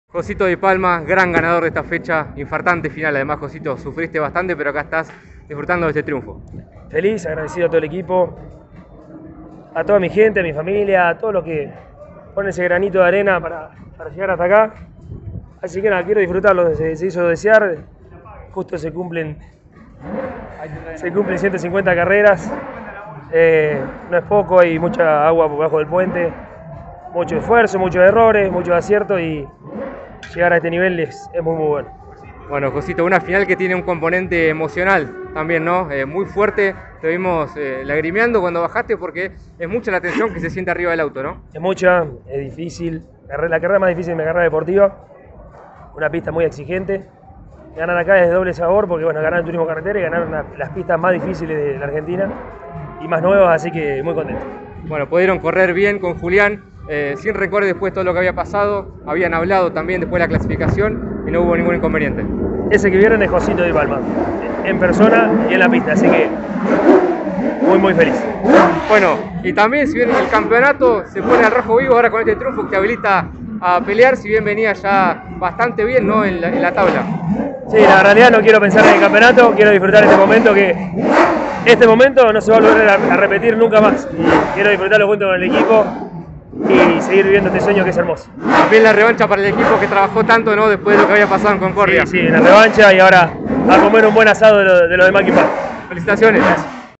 Josito Di Palma - Victoria Final F9 TC San Juan 2021
Luego de quedarse con la final de la novena fecha del TC en el autódromo de El Villicum, Josito Di Palma pasó por los micrófonos de Mundo Sport y comentó sus sensaciones por el resultado logrado.